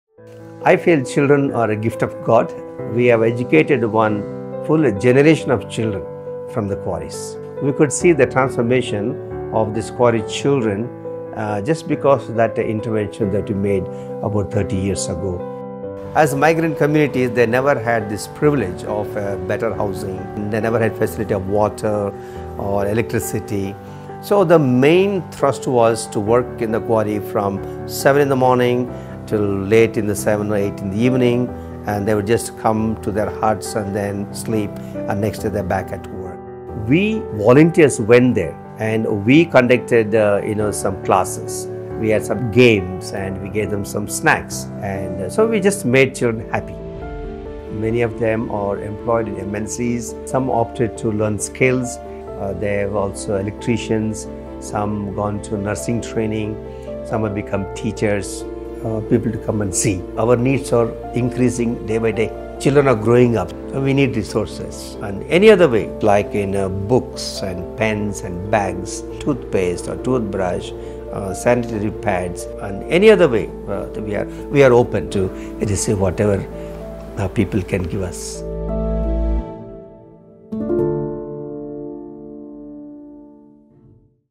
Join Hands With Thomas Swaroop And Help The Little Ones In Quarries
Thomas Swaroop has taken upon himself the task to give the underprivileged children of Bangalore quarries a better life. Support him in his efforts.